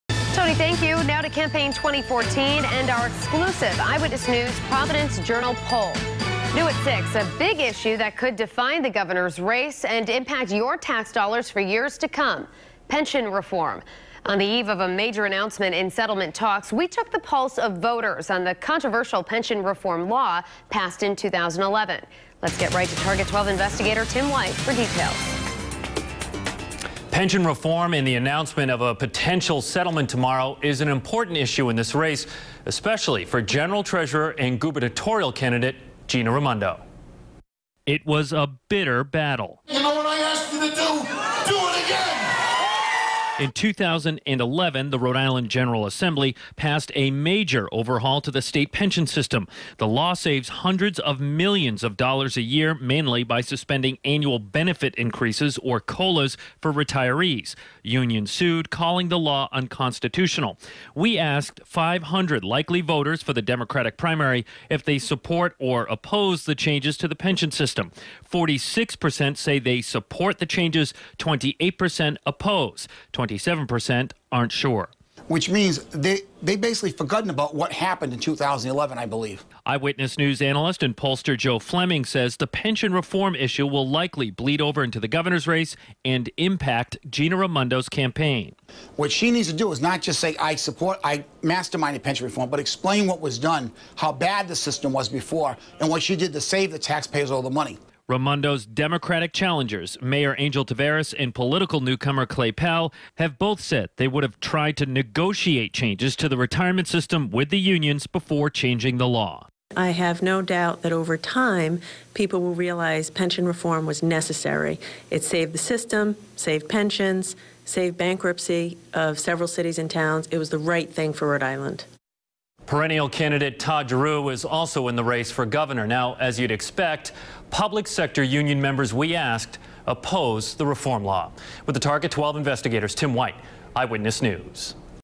WPRI/Journal Pension Poll
Tim White gives reports on polled opinion of the state pension reform.